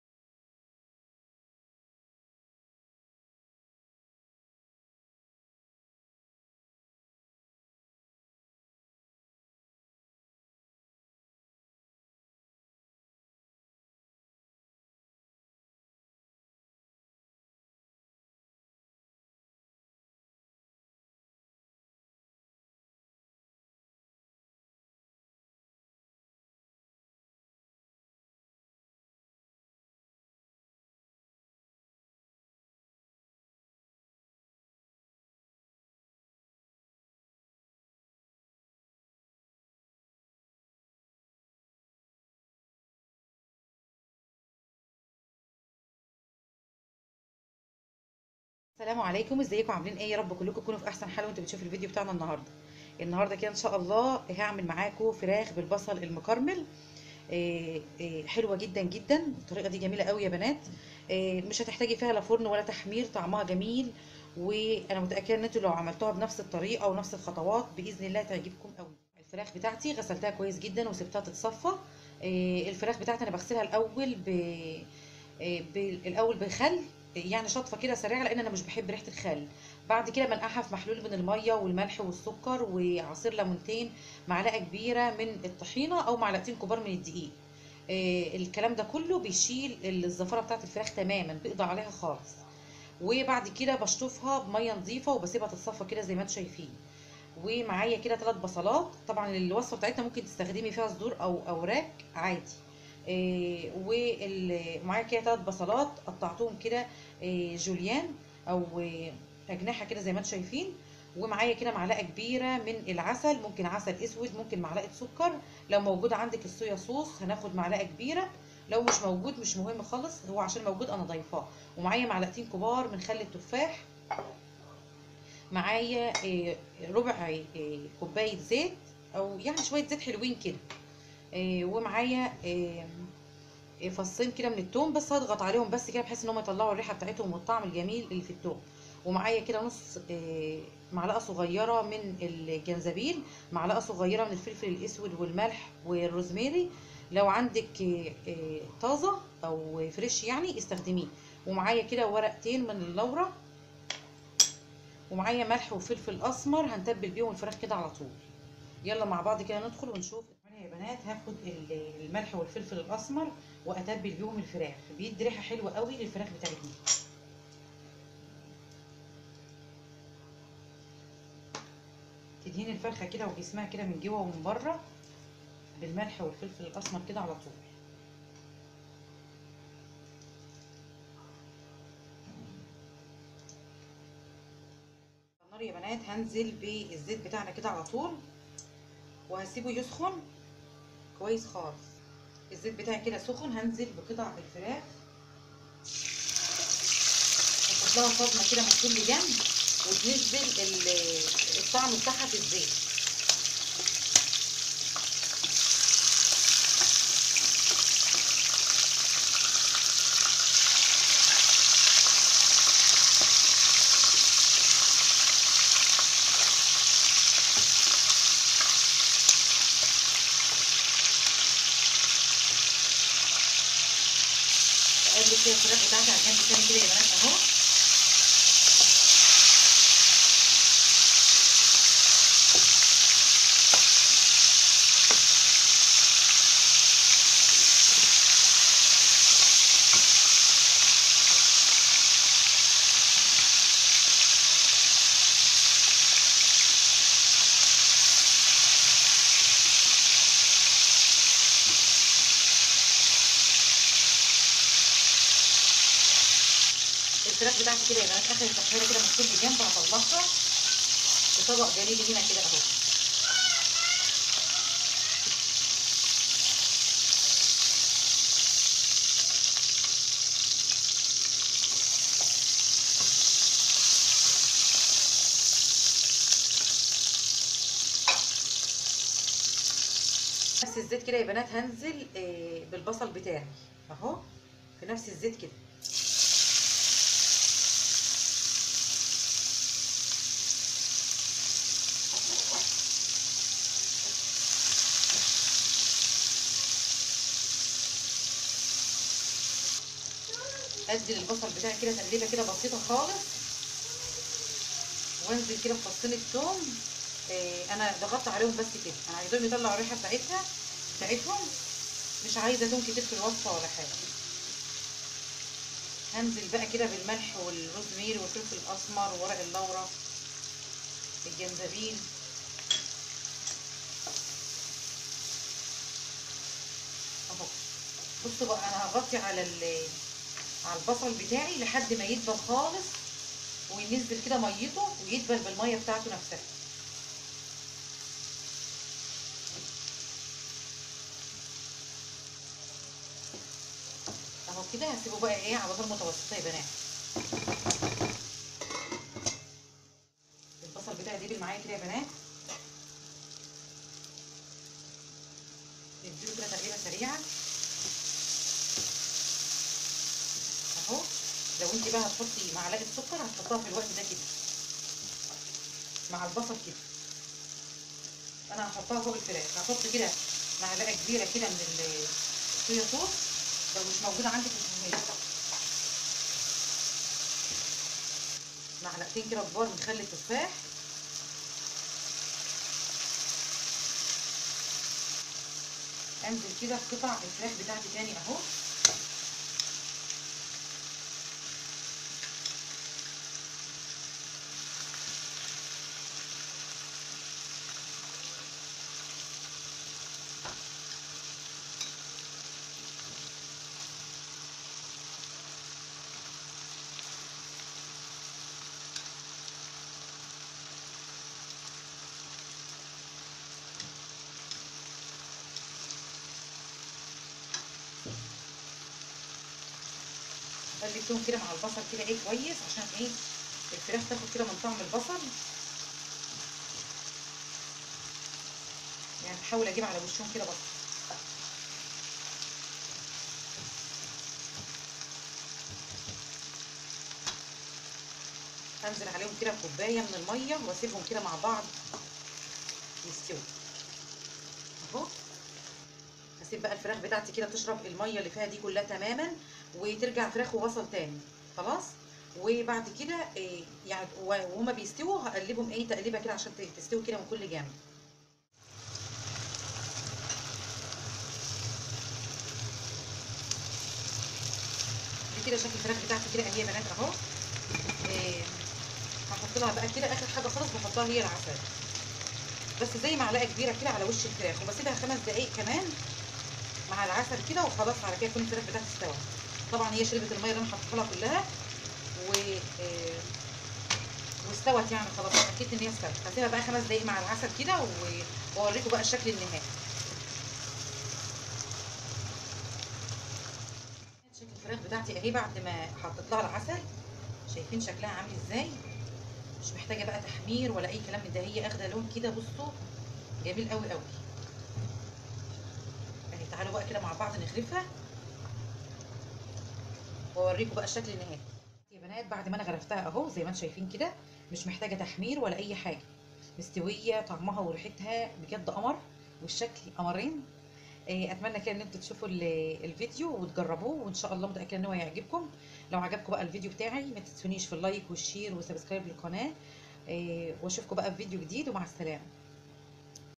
دجاج بالبصل (سويت اند ساور)
دجاج
بصل
2فص توم
2 ورق لاورا (غار)
م ك عسل نحل أو عسل اسود او م ك سكر
2 م ك صويا صوص
2 م ك خل التفاح
فلفل اسمر
ملح
جنزبيل بودر
روز ماري